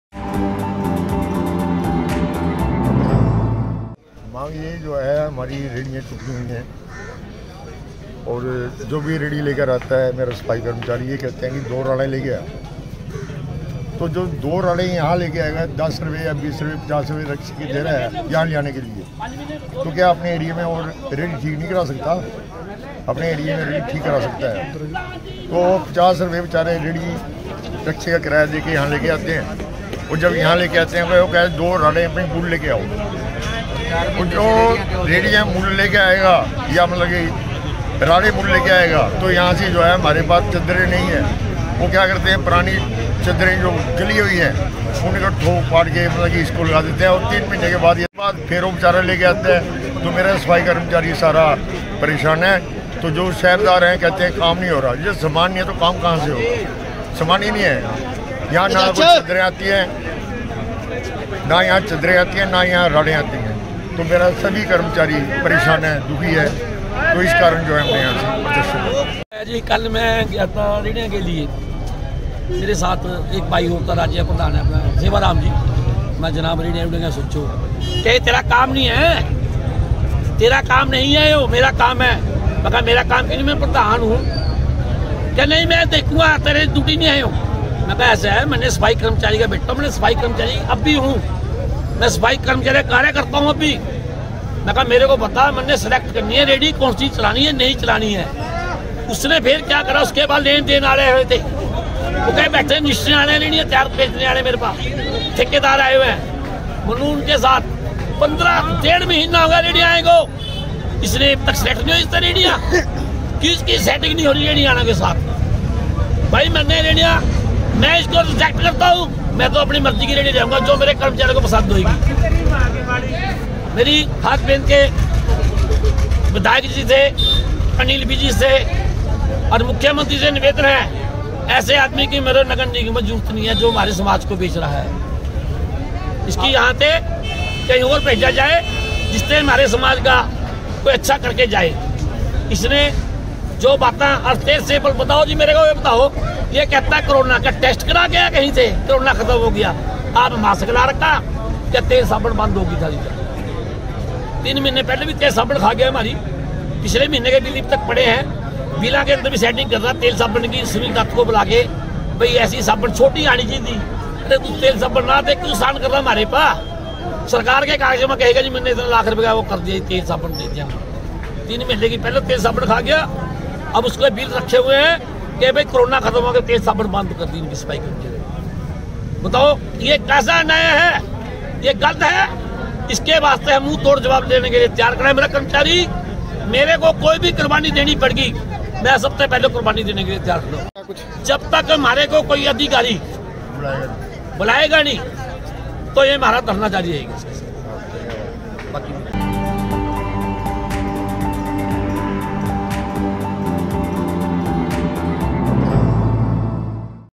सफाई के लिए उपकरण न मिलने से सफाई कर्मचारियों में रोष, निगम के गेट पर किया प्रदर्शन
shorturl.at/bzCFL